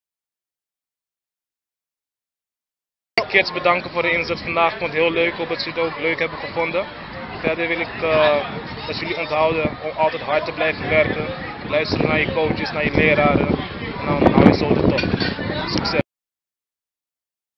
Made in Curacao: Kalian Sams
Curacao Baseball Week 2015 with Kalian Stams
His message to Curacao talents.

Kalian Rivalino Sams (Den Haag, 25 augustus 1986) is een Nederlandse honkballer van Curaçaose afkomst, die onder andere is verkozen tot Meest Waardevolle Speler bij Québec Capitales en bovendien verkozen is tot één van de Post-Season All Stars in de Can-Am League.